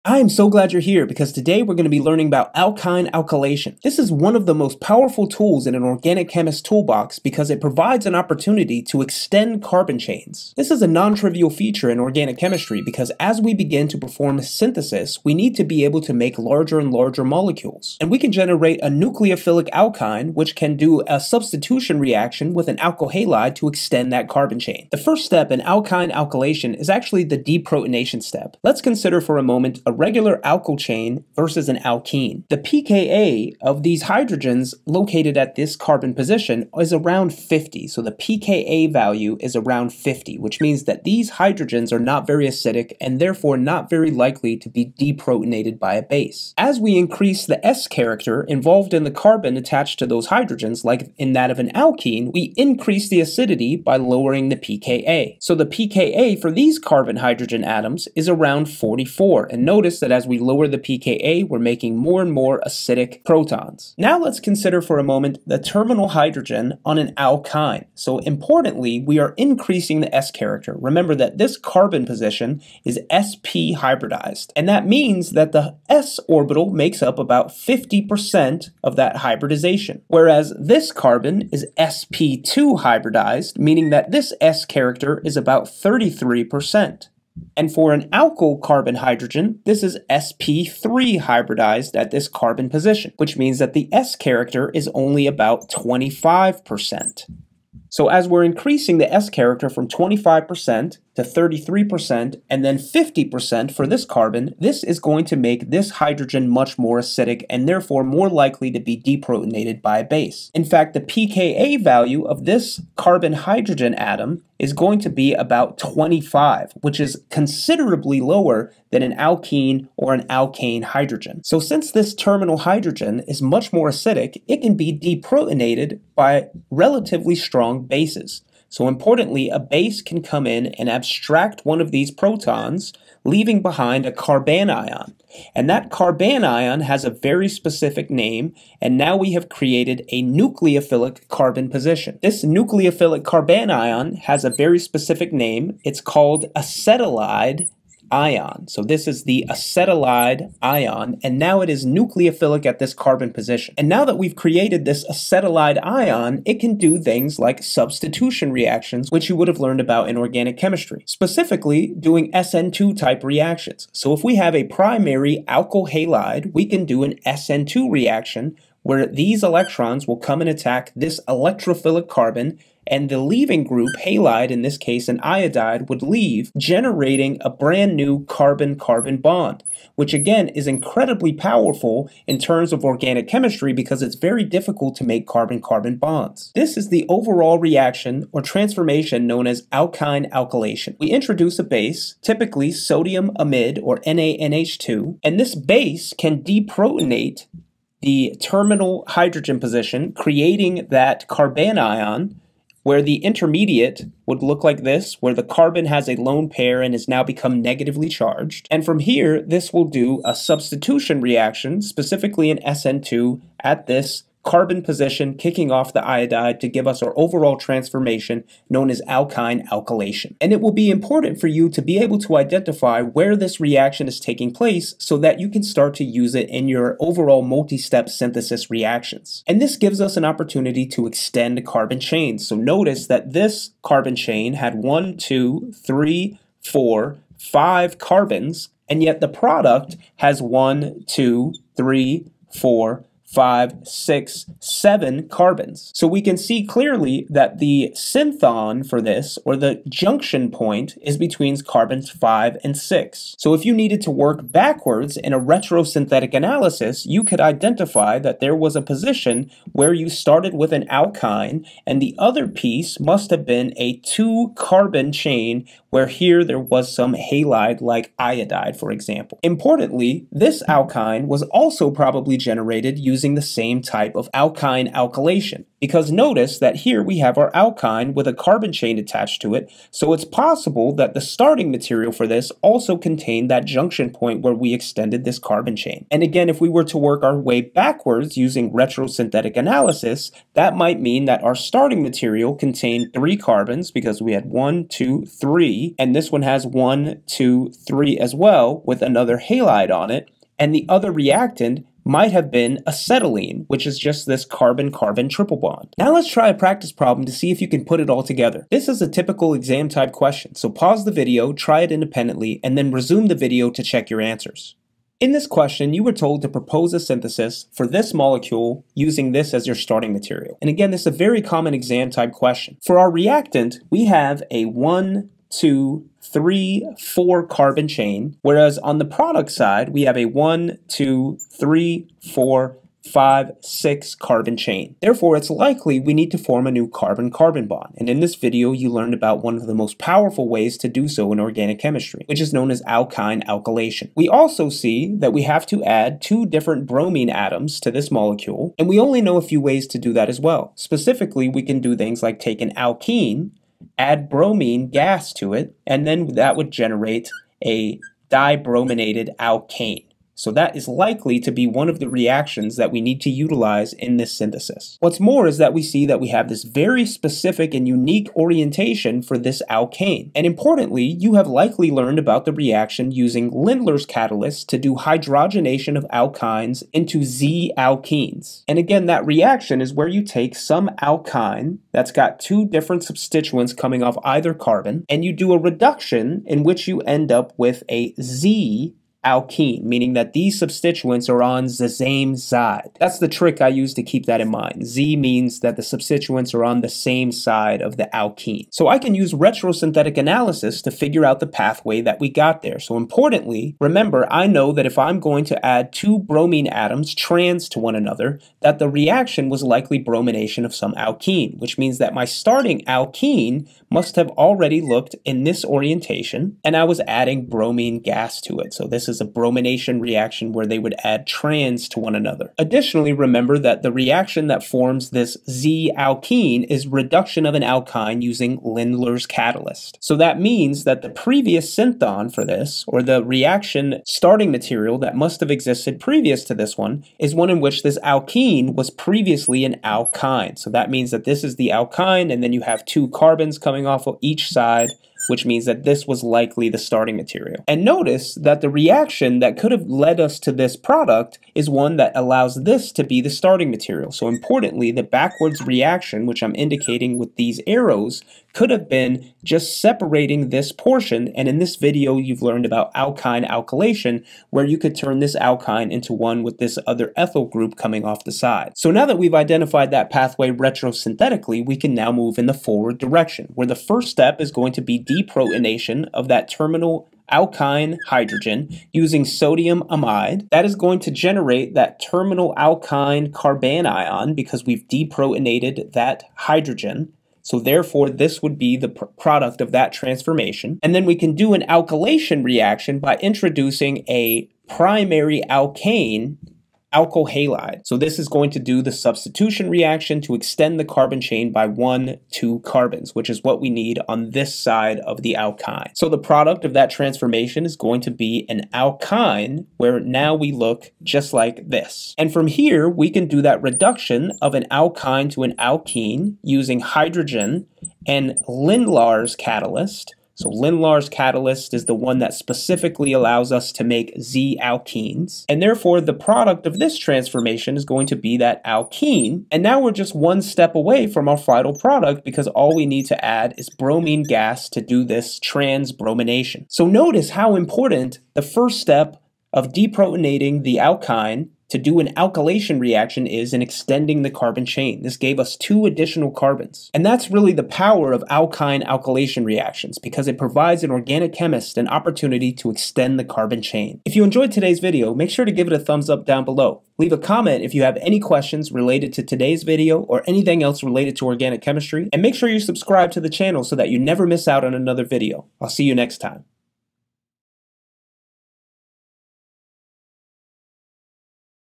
Alkyne Alkylation: How to Extend Carbon Chains in Organic Synthesis!
🧪 Need help with reactions?
💥 Instant download. Study smarter. Ace your exams.

Explore the detailed process of alkylation of terminal alkynes in this educational lightboard video! This step-by-step guide covers the mechanism, key considerations, and practical applications, making it perfect for students and chemistry enthusiasts. Learn how to extend carbon chains and create complex organic structures with ease.

📚 Chapters
0:00 - Introduction to alkylation and its significance in organic chemistry.
0:24 - Deprotonation: Generating the nucleophilic alkynide ion.
2:02 - Nucleophilic Substitution
3:07 - Example Mechanism: Detailed step-by-step of alkylating 1-butyne with methyl iodide.
5:05 - Practice Problems for Exam

Enhance your knowledge of synthetic organic chemistry by understanding the nuances of terminal alkyne alkylation.